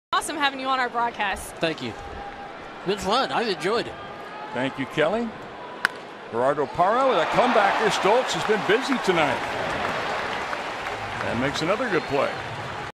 ARI@SD: Stults makes impressive catch from the mound
6/14/13: Eric Stults snatches Gerardo Parra's grounder, then fires to Kyle Blanks for an out in the top of the sixth inning